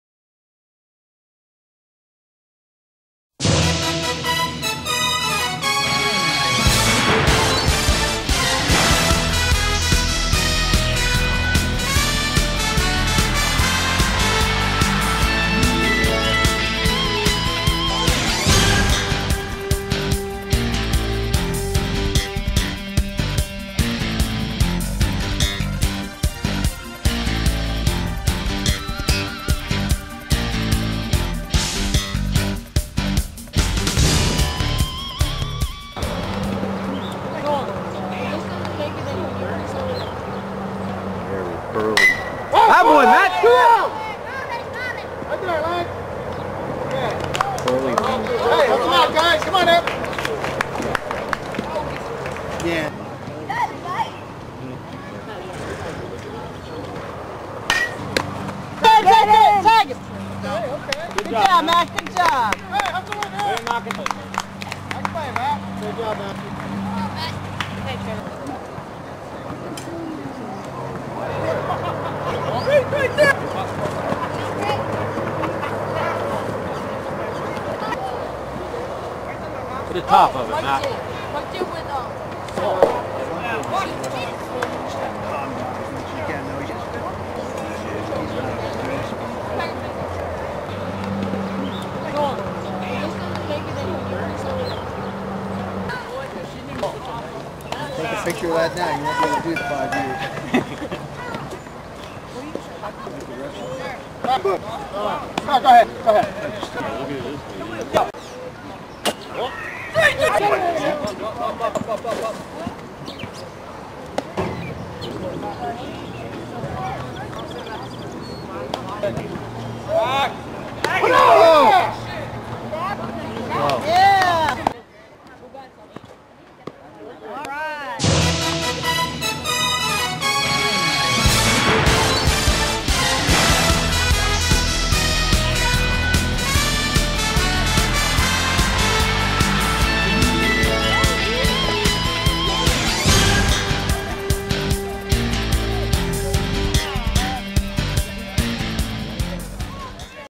Buggy Sports Network 5/17/2010
highlights from Kutztown Middle School's game vs. Reading Northeast on May 17, 2010